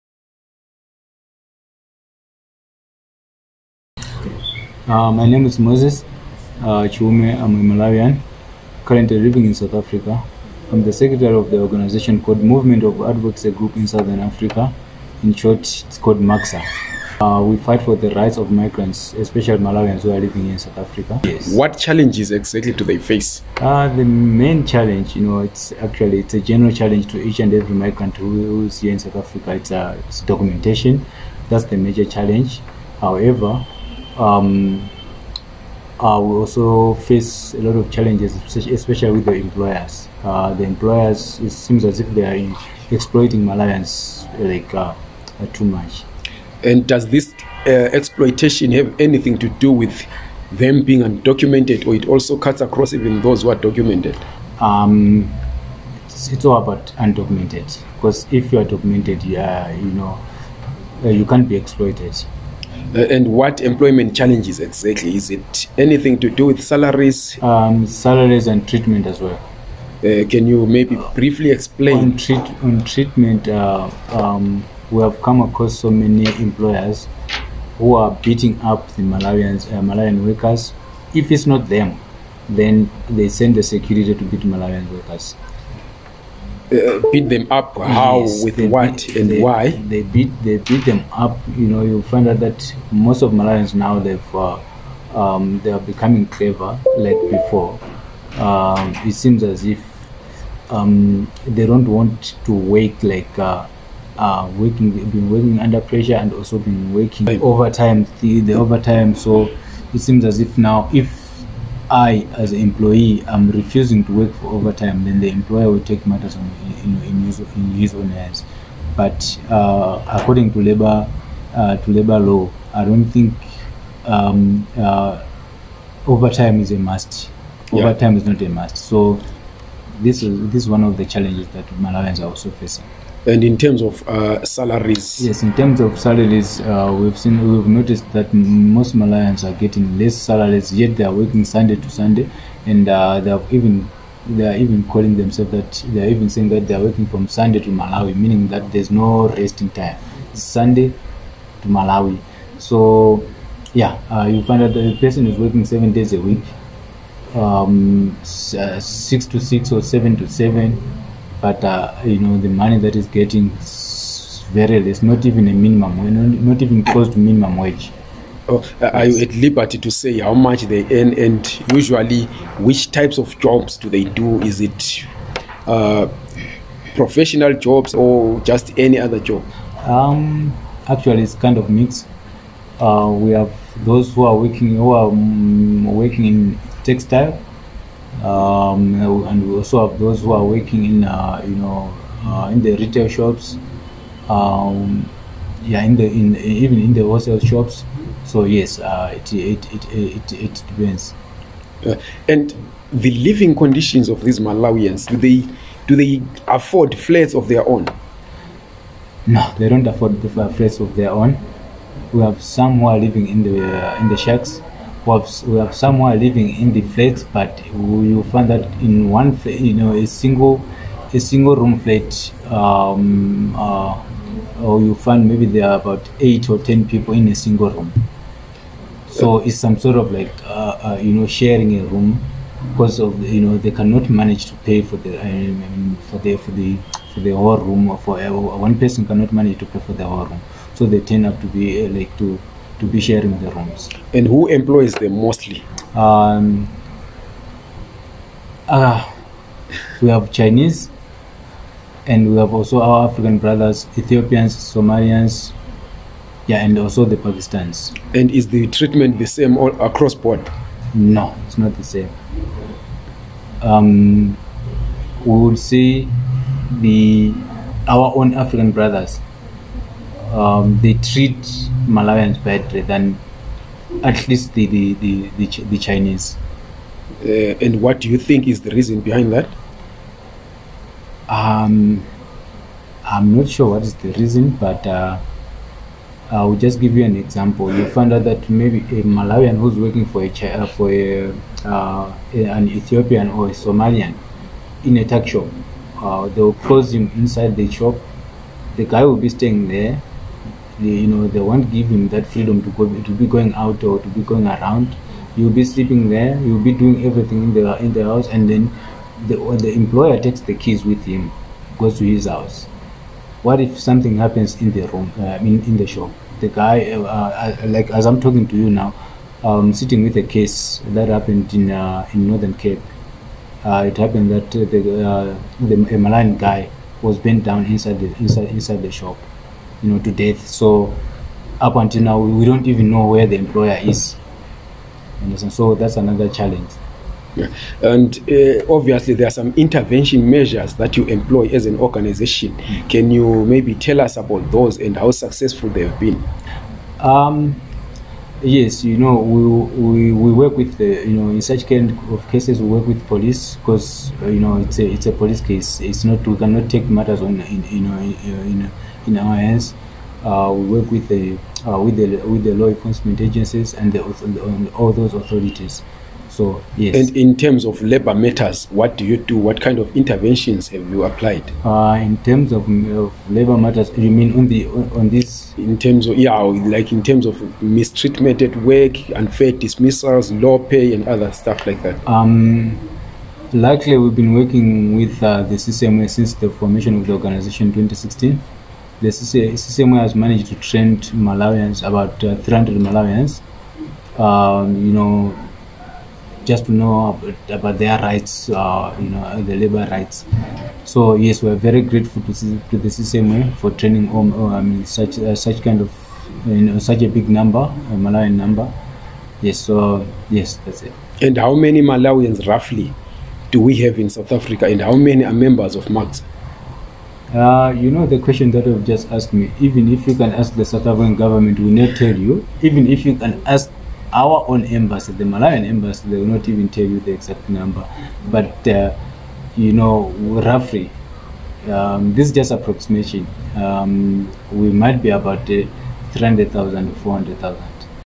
Malawian workers in South Africa: A story of exploitation, low wages and crammed-up living. CCMA
MAGSA Secretary, Moses Chiume, speaks to AVG News on the challenges facing Malawian workers in South Africa.